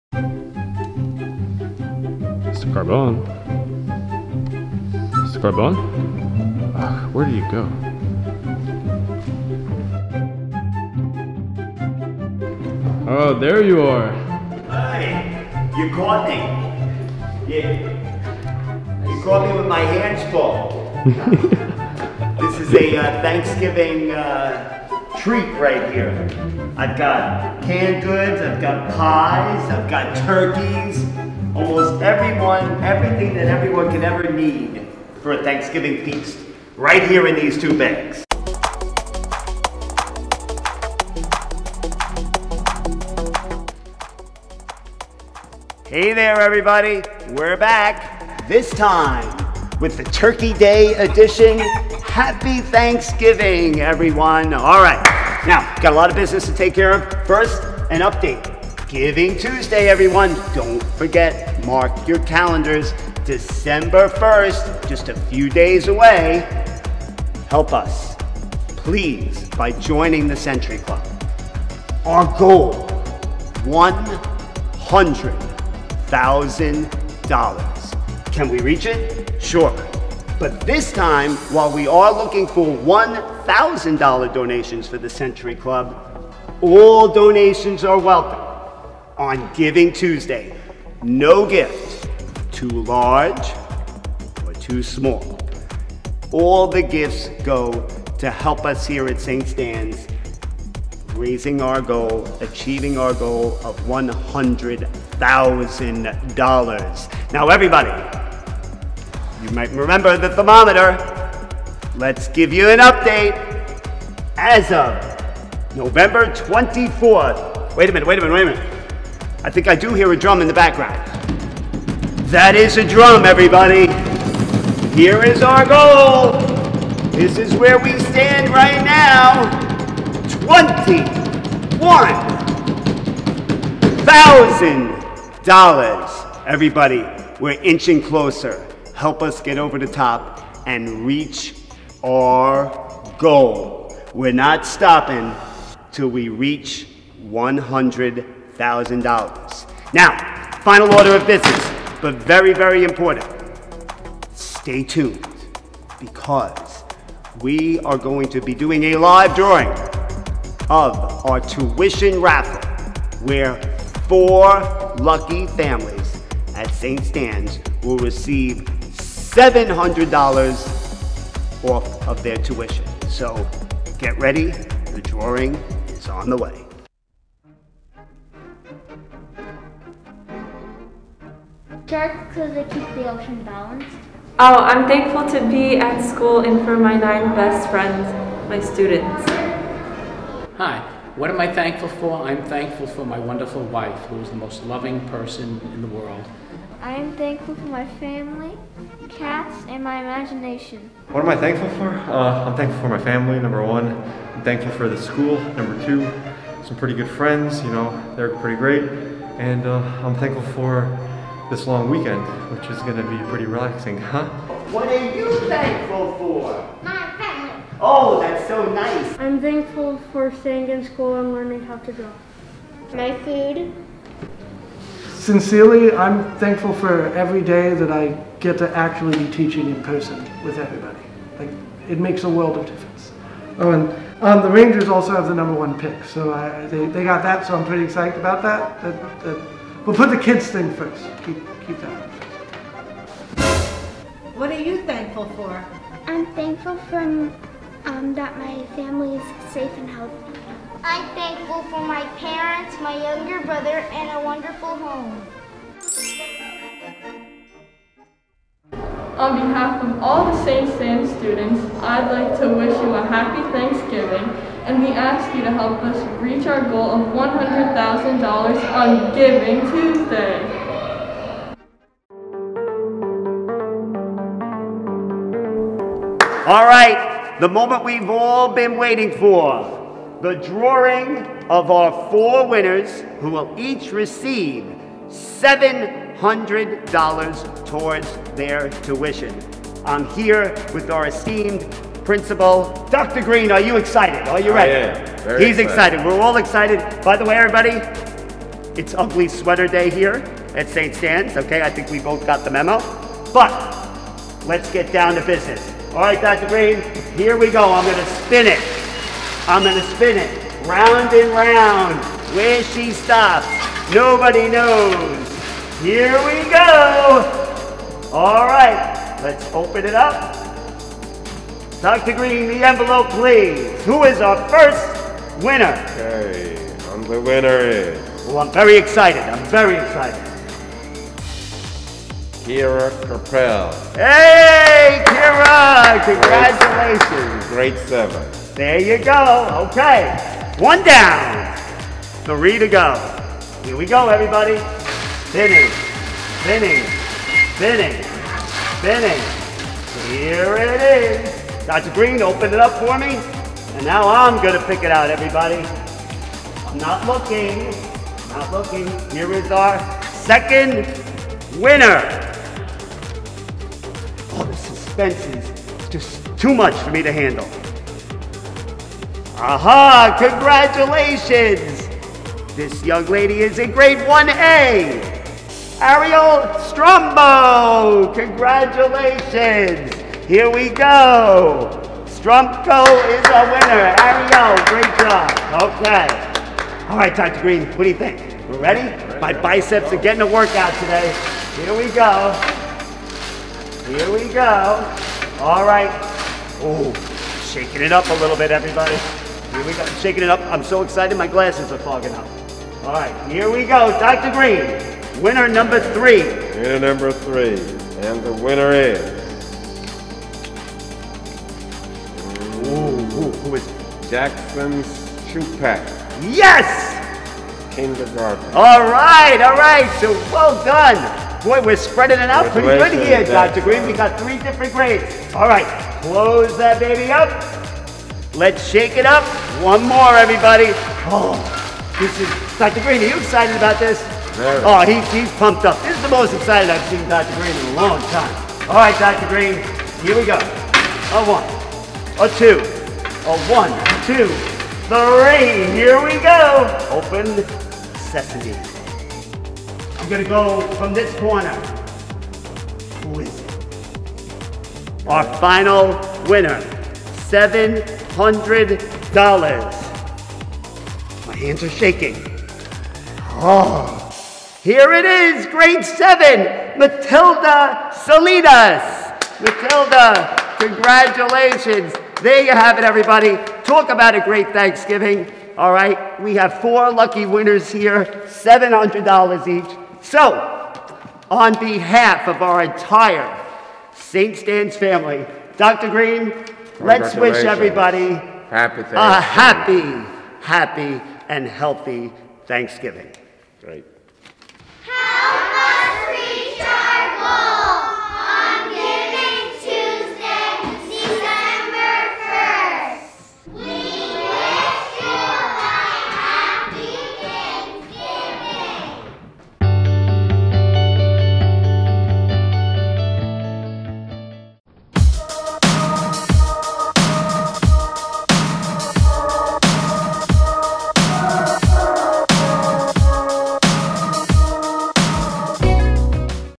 SSKCA Thanksgiving Day Special: Giving Thanks + Tuition Raffle Drawing
Come join the St. Stan's family and participate in our innovative fundraiser! Our goal is $100,000!! Help us reach our goal!